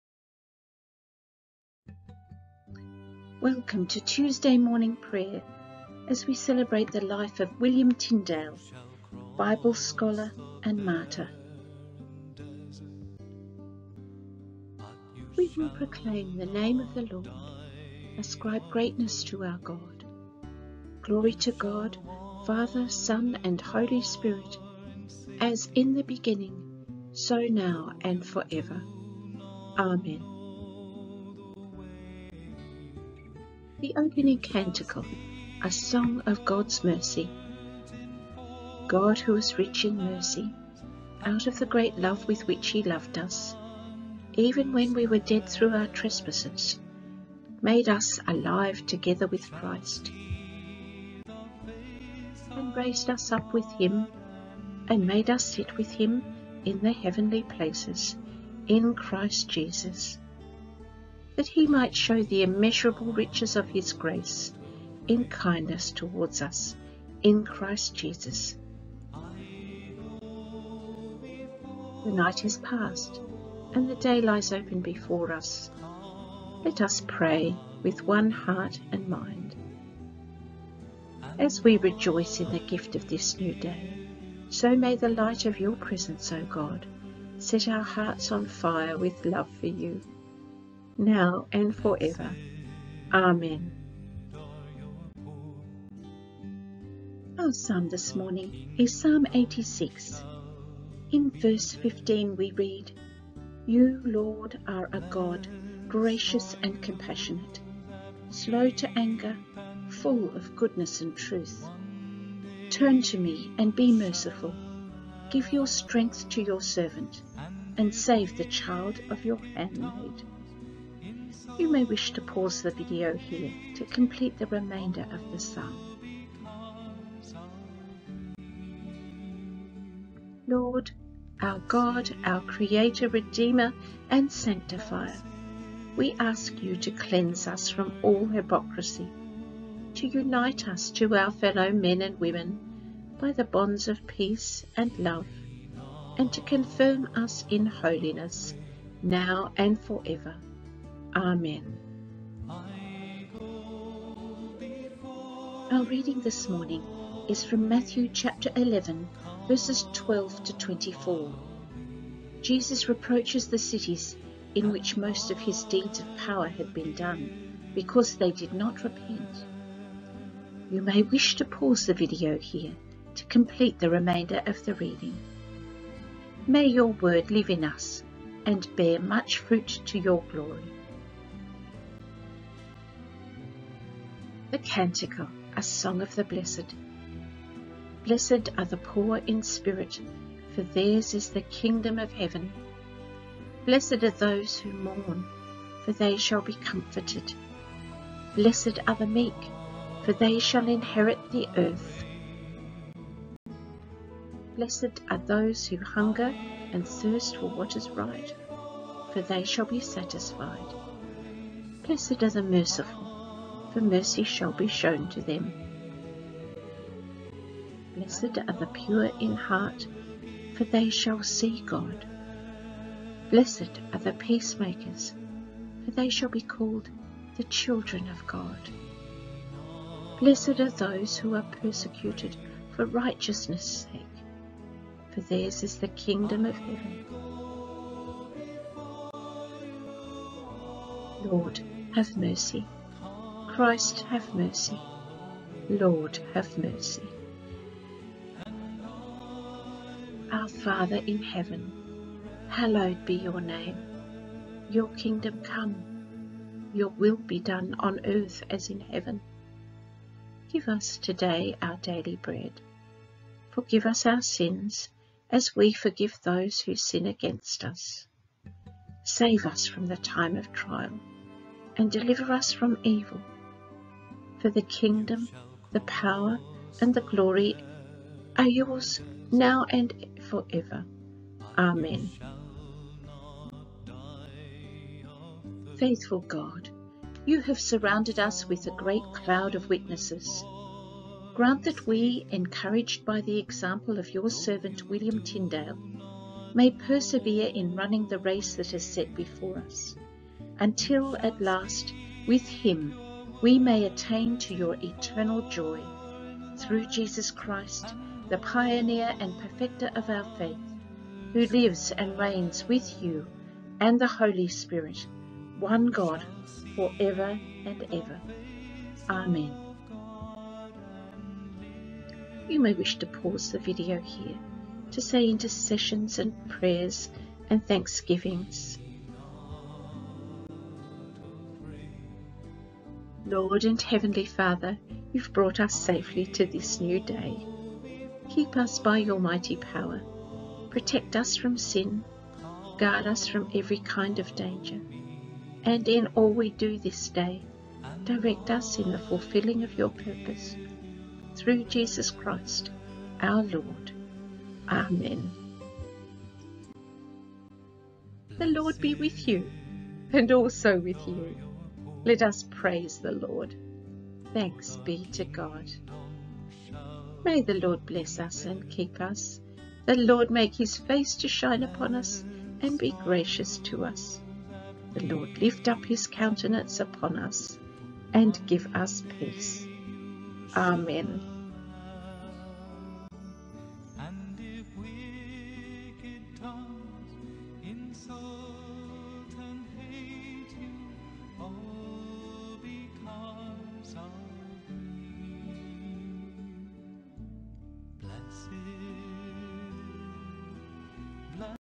Tuesday Morning Prayer 20
William Tyndale, (born c. 1490–94, near Gloucestershire, England—died October 6, 1536, Vilvoorde, near Brussels, Brabant), English biblical translator, humanist, and Protestant martyr. A number of partial English translations had been made from the 7th century onwards and Luther's German Bible had appeared in 1522. Tyndale responded to a fellow clergyman at the time: "I defy the Pope, and all his laws; and if God spares my life, ere many years, I will cause the boy that driveth the plow to know more of the Scriptures than thou dost!"
Forbidden to work in England, Tyndale translated and printed in English the New Testament and half the Old Testament between 1525 and 1535 in Germany and the Low Countries. He worked from the Greek and Hebrew original texts when knowledge of those languages in England was rare.
His pocket-sized Bible translations were smuggled into England, and then ruthlessly sought out by the Church, confiscated and destroyed.
The religious ferment caused by Wycliffe's Bible in the late 14th century led to the death penalty for anyone found in unlicensed possession of Scripture in English. Condemned as a heretic, Tyndale was strangled and burned outside Brussels in 1536.
In 1611, the 47 scholars who produced the King James Bible drew significantly from Tyndale's original work. One estimate suggests that the New Testament in the King James Version is 83% Tyndale's words and the Old Testament 76%. Hence, the work of Tyndale continued to play a key role in spreading Reformation ideas across the English-speaking world and eventually across the British Empire.
Gracious God, may your word live in us and bear much fruit to your glory.
Rev’d Mandy

Readings:
Psalm 86
Matthew 11.12-24